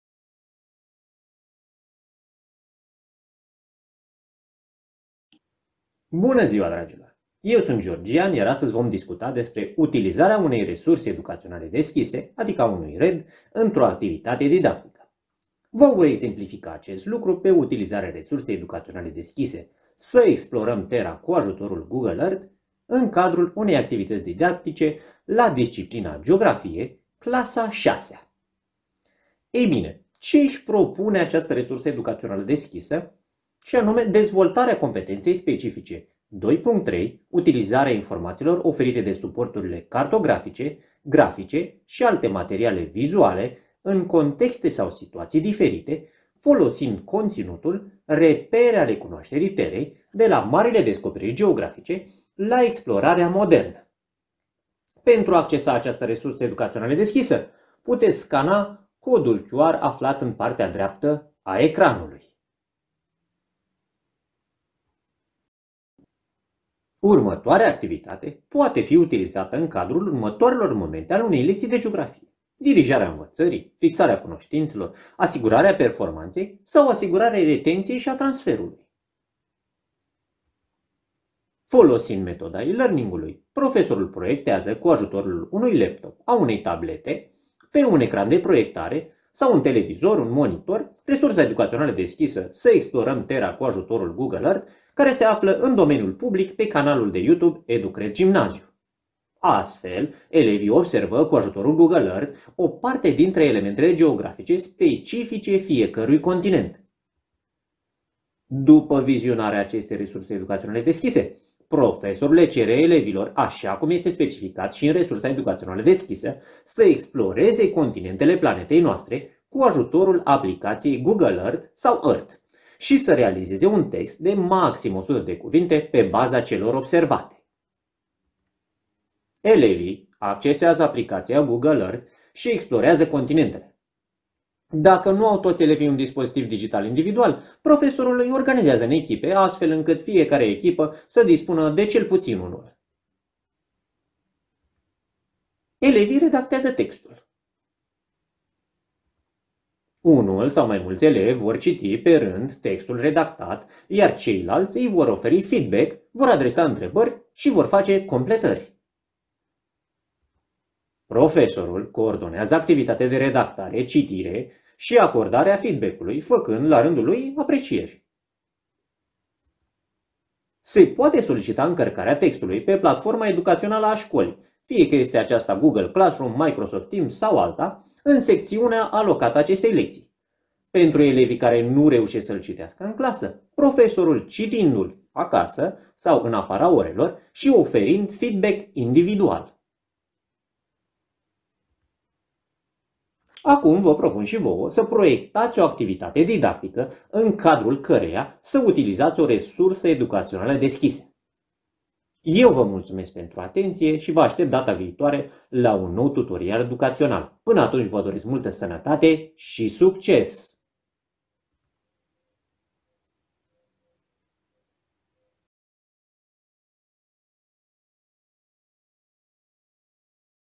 Proiectarea unei activități didactice folosind o resursă educațională deschisă. Exemplul 1
În cadrul acestui tutorial este prezentat un exemplu de utilizare a unei resurse educaționale deschise în cadrul unei activități didactice.
Ca exemplu a fost aleasă resursa educațională deschisă „ Să explorăm Terra cu ajutorul Google Earth” care se află în domeniul public,
pe canalul de youtube „EDUCRED Gimnaziu” 70Vw).
Resursa este la disciplina geografie, clasa a VI-a, vizând dezvoltarea competenței specifice 2.3. Utilizarea informaţiilor oferite de suporturile cartografice, grafice şi alte materiale vizuale în contexte/situaţii diferite, din programa școlară la disciplina geografie.